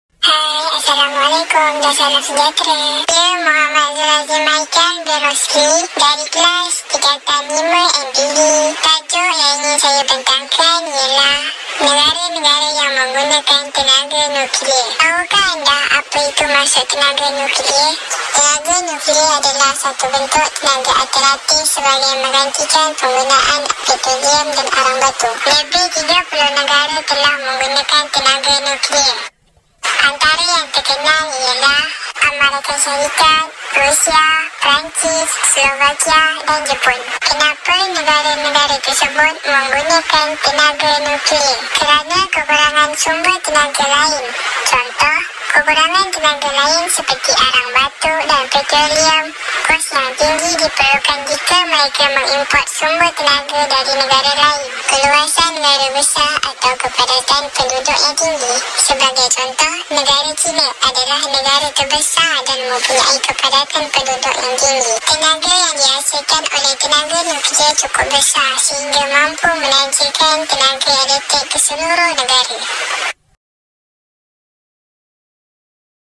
Pembentangan 7 I Tenaga Nuklear I Zulhazim Haikal I 2021 I#PBS
Zulhazim Haikal Rosli telah membuat pembentangan ke 7.Mohon beri sokongan kepada Zulhazim dengan like . komen dan share.